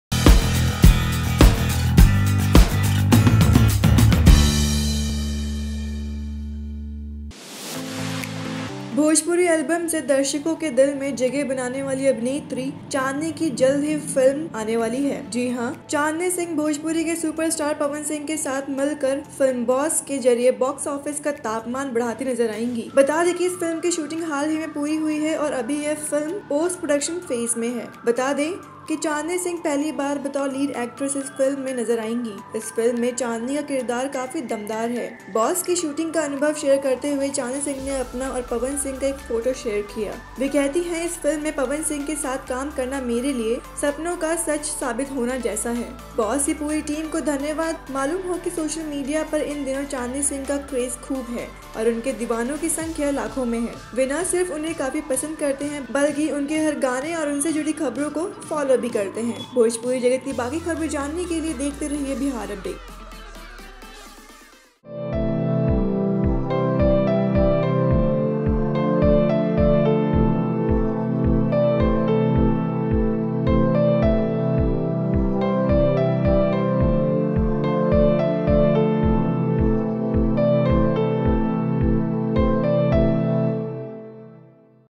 Boss में Chandani Singh पहली बार लीड एक्‍ट्रेस बनी |Bihar Update
भोजपुरी अलबम से दर्शकों में दिल में जगह बनाने वाली अभिनेत्री  Chandani Singh की जल्द ही फिल्म में नज़र आने वाली है। जी हाँ, चांदनी सिंह bhojpuri के सुपर स्‍टार Pawan Singh के साथ मिलकर फिल्‍म ‘बॉस’ के जरिये बॉक्‍स ऑफिस का तापमान बढ़ाती नजर आयेंगी। बता दें कि इस फिल्‍म की शूटिंग हाल ही में पूरी हुई है और अभी यह फिल्म पोस्‍ट प्रोडक्‍शन फेज में है।

बता दें की,  Chandani Singh पहली बार बतौर लीड एक्‍ट्रेस इस फिल्म में नजर आयेंगी। इस फिल्‍म में  Chandani Singh का किरदार काफी दमदार है। बॉस की शूटिंग का अनुभव शेयर करते हुये चांदनी सिंह ने अपना और पवन सिंह का एक फोटो शेयर किया। वे कहती हैं इस फिल्म में पवन सिंह के साथ काम करना मेरे लिये सपनों को सच साबित होने जैसा है। बॉस की पूरी टीम को धन्यवाद। मालूम हो कि सोशल मीडिया पर इन दिनों  Chandani Singh का क्रेज खूब है और उनके दीवानों की संख्‍या लाखों में हैं। वे न सिर्फ उन्हें काफी पसंद करते हैं, बल्कि उनकी हर गाने और उनस जुड़ी खबरों को फॉलो भी करते हैं।